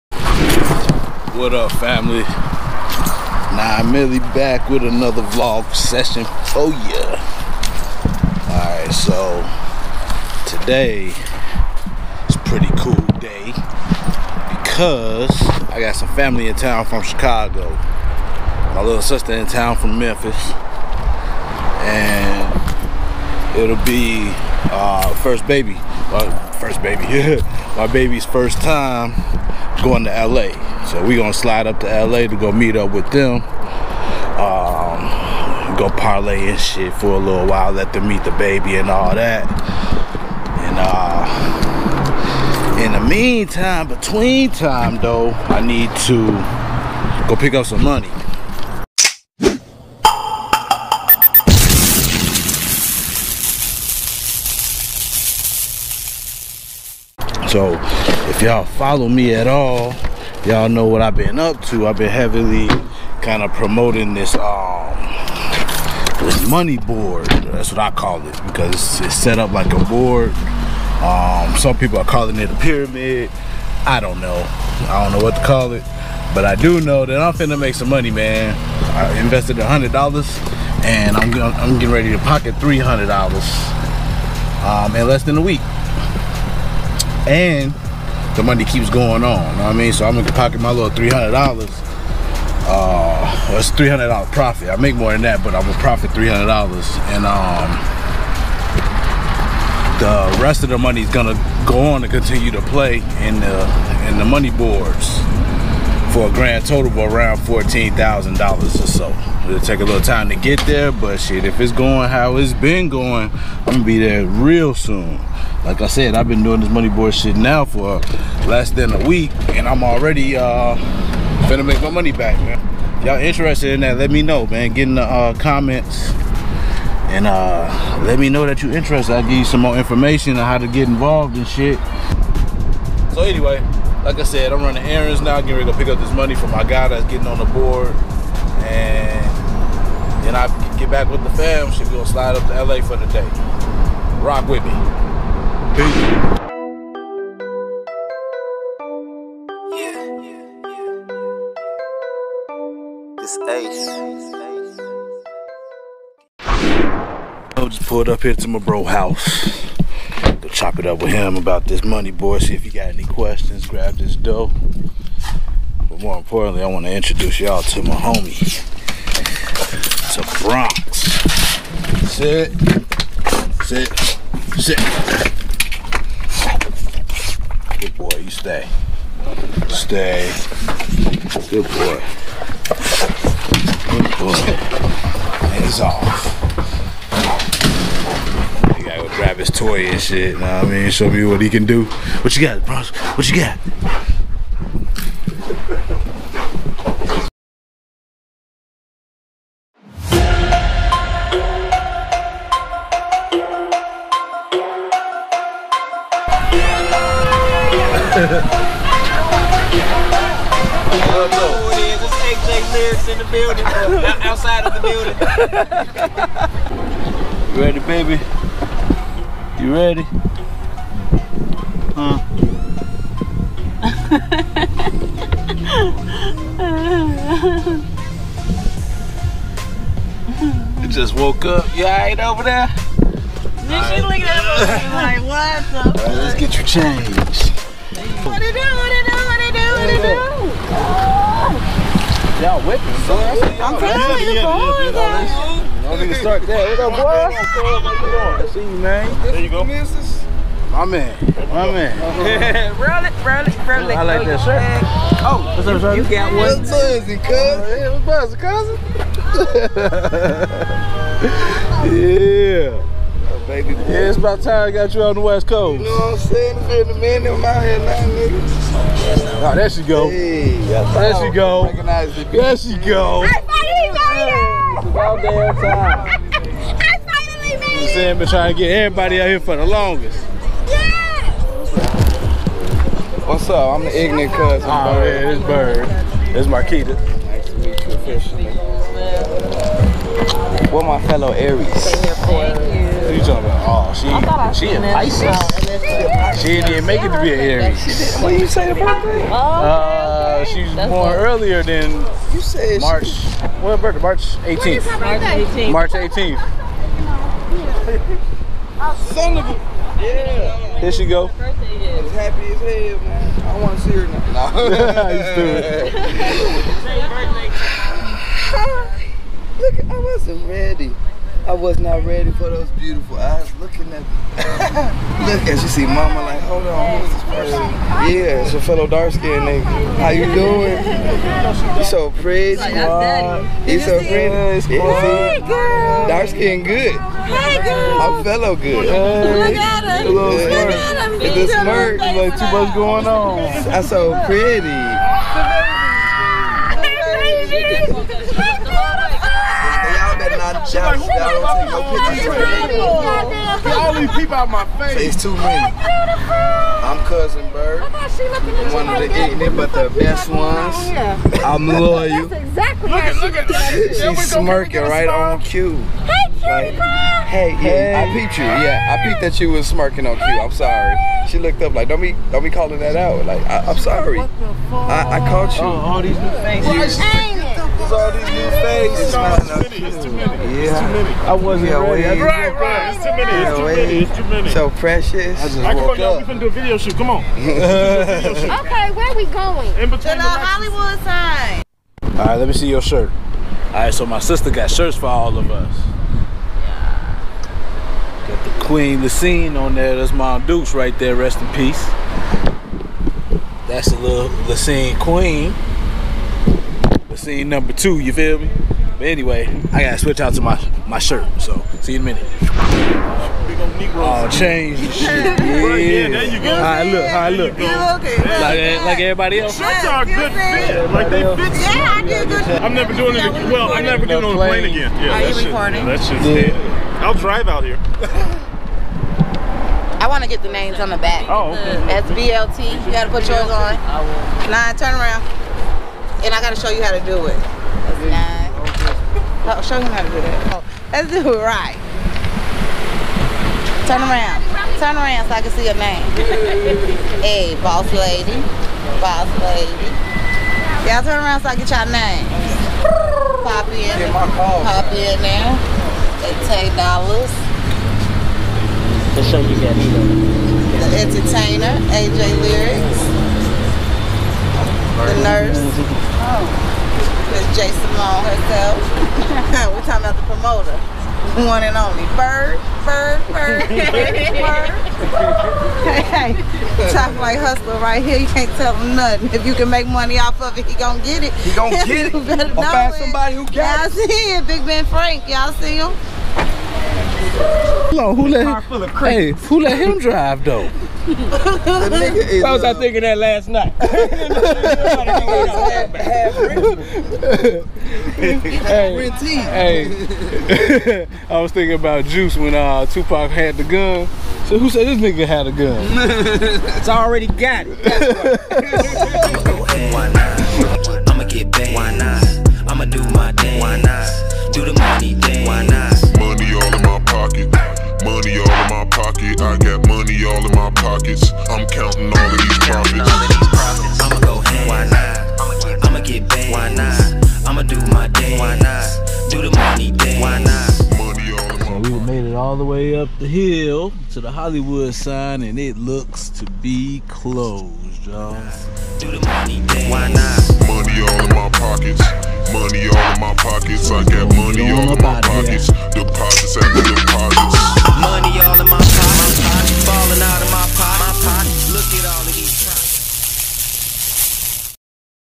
Newborn Baby 1st L.A. trip &Hollywood Sign with the Family
My sister and cousins came to  Hollywood California
so I took my lady and baby to meet them.